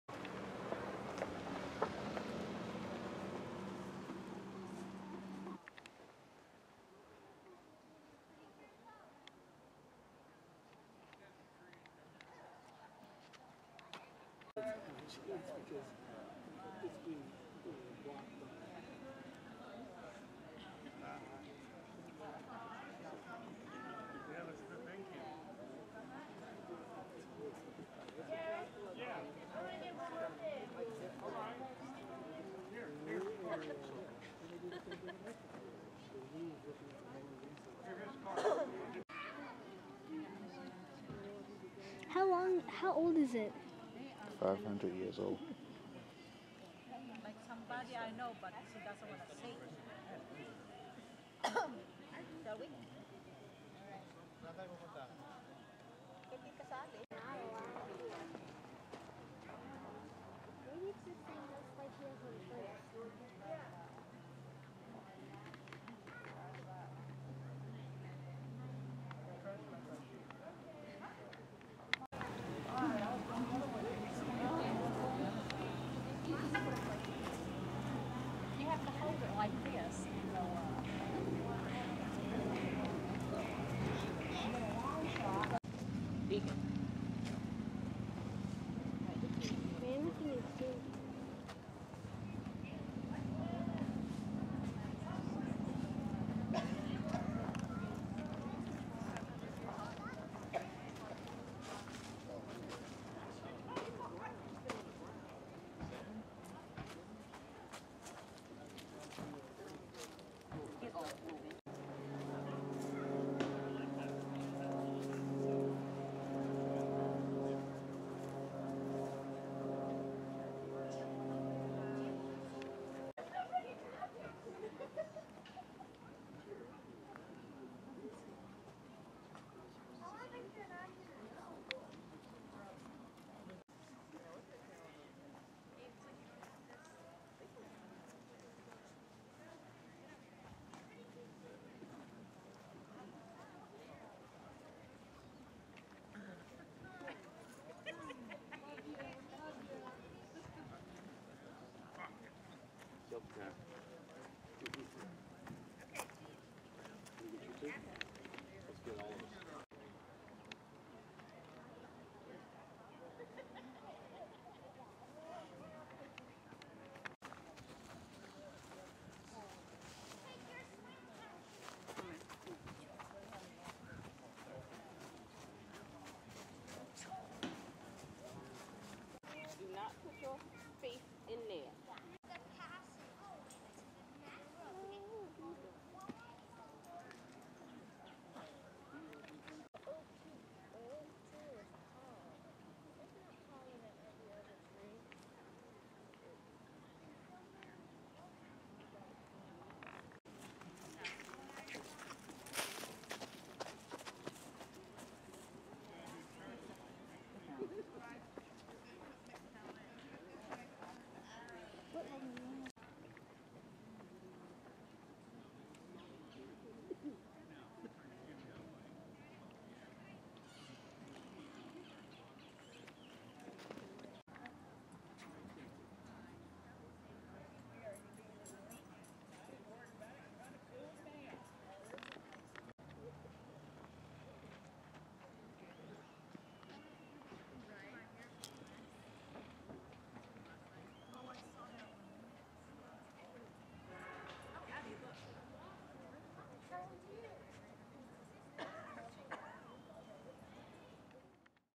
Travel to The 500 Year Old Angel Oak Tree in Johns island, SC!
This video is about angel oak in Johns Island SC! Angel Oak is a Southern live oak (Quercus virginiana) located in Angel Oak Park on Johns Island near Charleston, South Carolina. The tree is estimated to be 400-500 years old.[1] It stands 66.5 ft (20 m) tall, measures 28 ft (8.5 m) in circumference, and produces shade that covers 17,200 square feet (1,600 m2). Its longest branch distance is 187 ft[2] in length. Angel Oak was the 210th tree to be registered with the Live Oak Society.